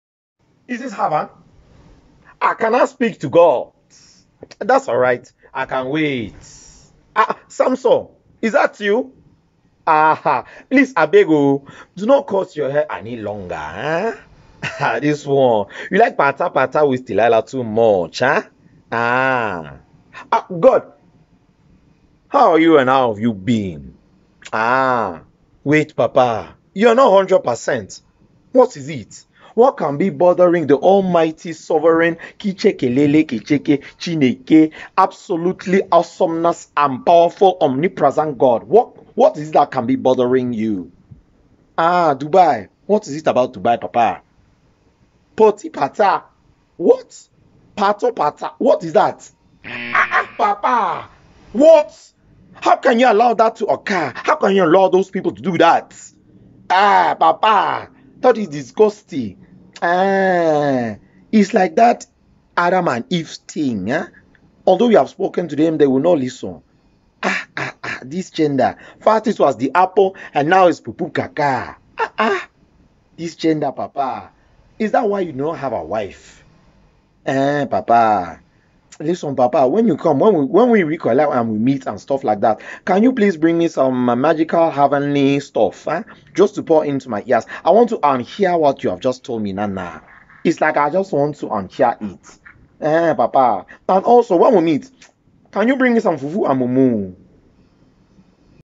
Pastor Egbe Calls Heaven to talk to God... about Dubai! 💩👱🏾‍♀️
Pastor Egbe has a direct line to hevean! 📲

He calls GOD'ooo, to ask how he is. He's surprised to find out that God is not ok... Something to do with Dubai. 🤷🏽‍♂️